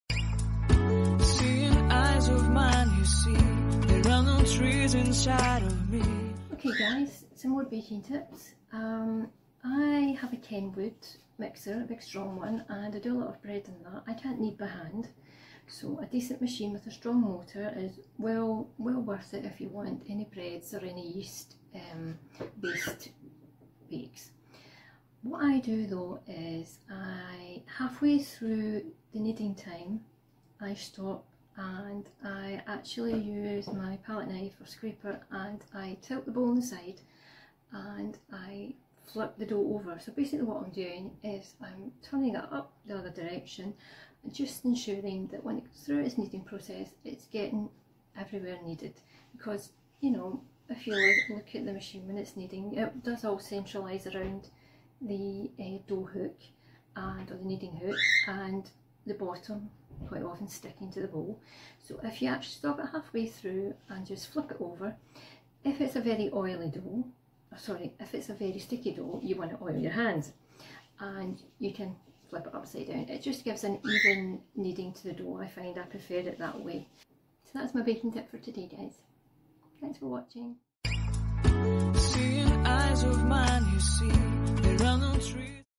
KNEADING DOUGH BY MACHINE - Baking Tips
Caroline's Easy Baking Lessons shared more baking tips, this time regarding what she does when kneading bread dough by machine.

I am a self-taught baker and want to share my knowledge with everyone.
Caro x